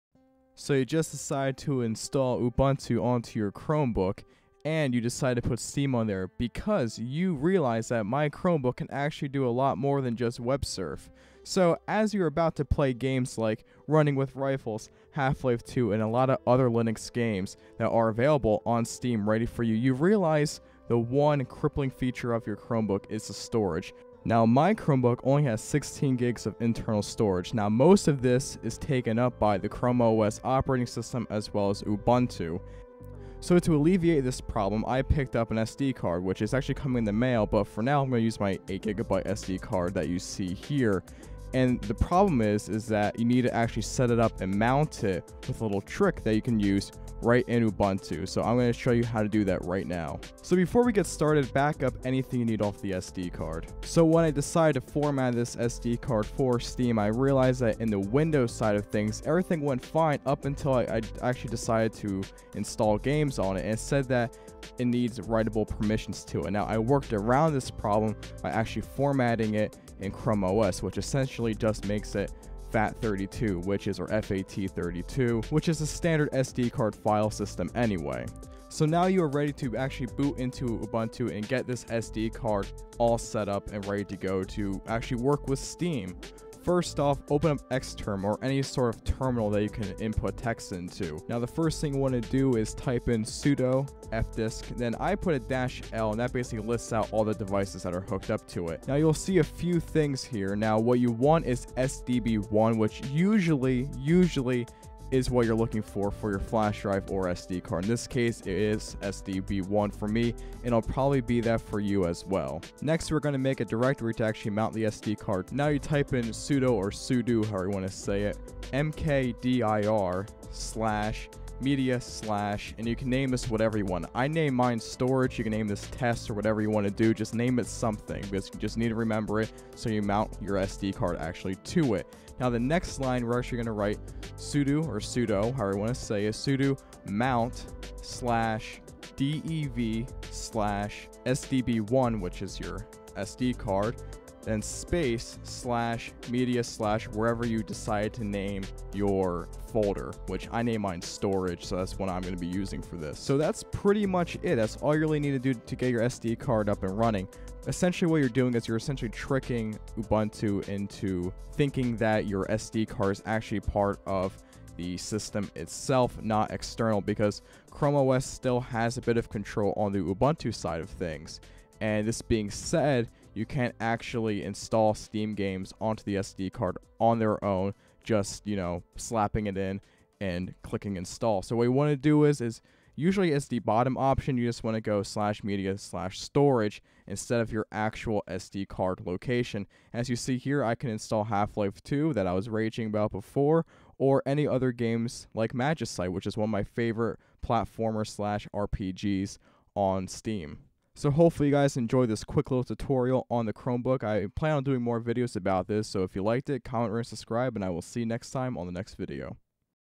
How to Install Steam Games to a SD Card/Flash Drive on a Chromebook
Also one little goof i noticed when editing was that i mounted sd card to the storage folder instead of the test folder that we made, this is because i already made the storage folder previously before recording this video!